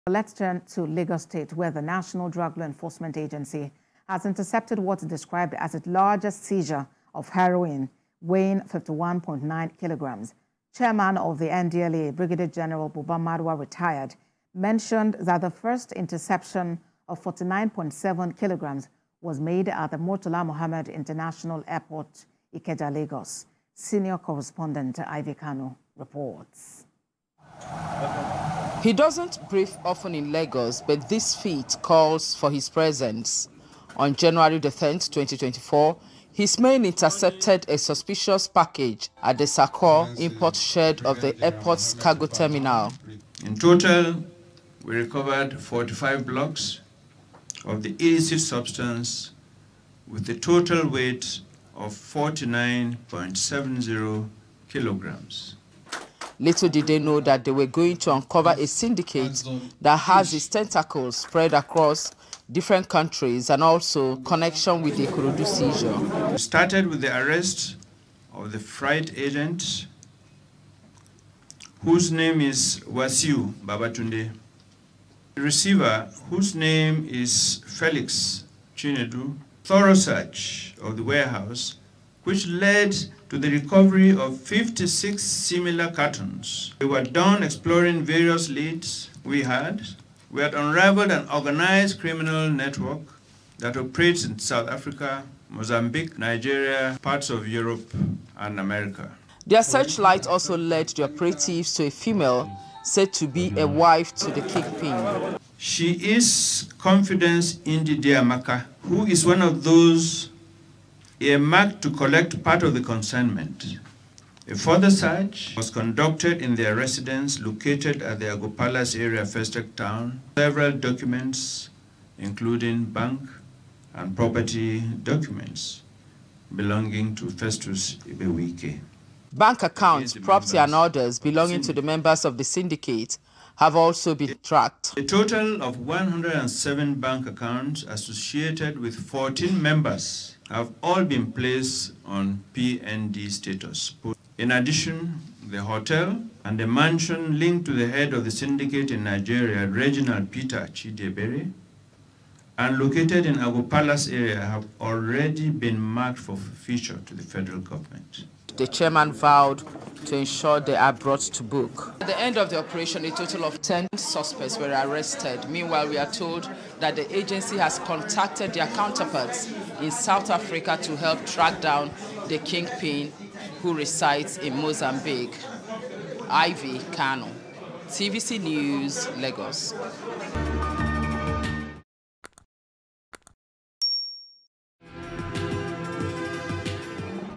NDLEA Makes Largest Seizure Of 51.9 KG Heroin At Lagos Airport
The National Drug Law Enforcement Agency, NDLEA, has intercepted what it described as its largest seizure of heroin weighing 51.90kg.

Chairman of the NDLEA, Brigadier General Buba Marwa (Retired) mentioned that the first interception of 49.70kg was made at the Murtala Mohammed International Airport, MMIA Ikeja Lagos.